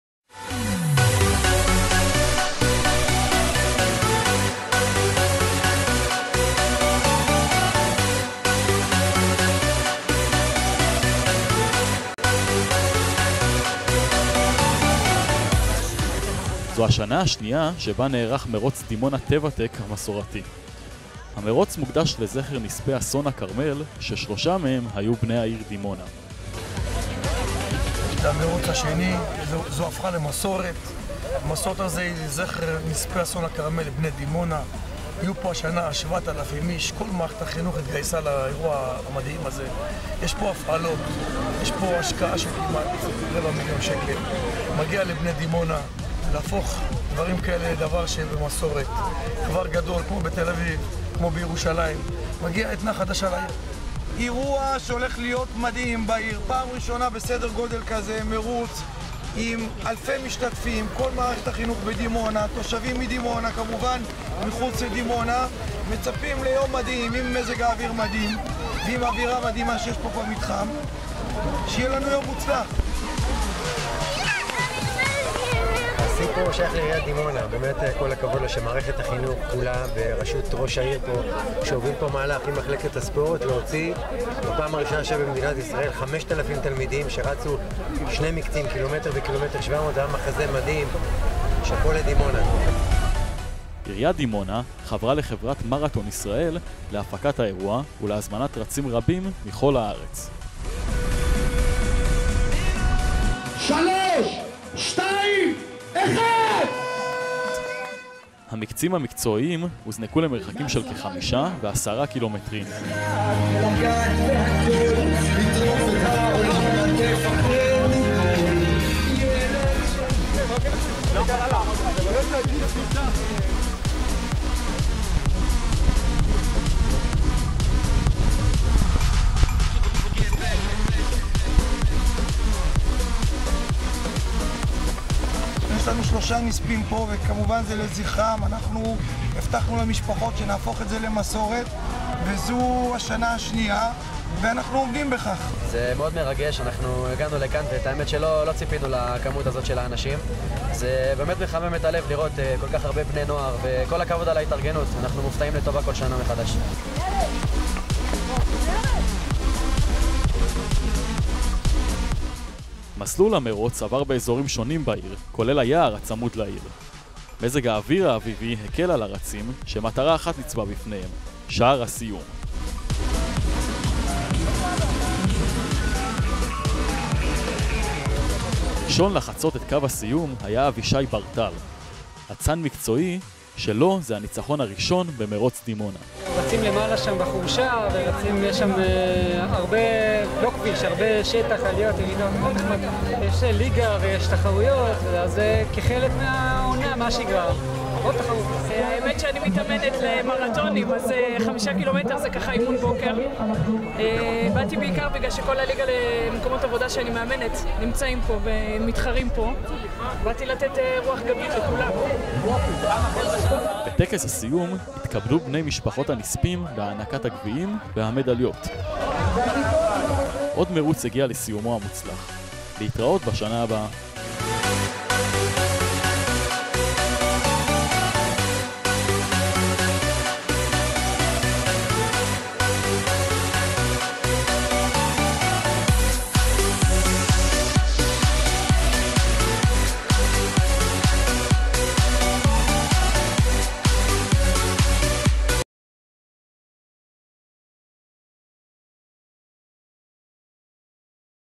מרוץ דימונה טבע טק 2015 לזכר נספי אסון הכרמל Dimonarun
מרוץ דימונה טבע טק ה-3 לזכר נספי אסון הכרמל, יערך ב 11.3.16 נפגשים
בגן יד לבנים, רחוב הרצל מול התחנה המרכזית בדימונה.
מסלול תחרותי למרחקים 10 ק"מ ו- 5 ק"מ
מסלול עממי למרחק 2 ק"מ – חינם

פסטיבל חגיגי לכל המשפחה
אירוע חגיגי וססגוני במתחם הכינוס:
הפעלות לילדים
פארק מתנפחים
שולחנות משחק
הפעלת זומבה וקיקבוקס
רקדניות ברזילאויות שילוו את הרצים
ספורט אתגרי
ריקודי ברייקדנס

06:30-08:15 חלוקת ערכות במתחם הזינוק
8:20 מקצה 5 ק"מ
8:25 מקצה 10 ק"מ
09:50 עממי בתי ספר כיתות א-ו
10:00 עממי בתי ספר כיתות ז-ט
10:15 מקצה 2 ק"מ עממי וכיתות יב
10:30 טכס סיום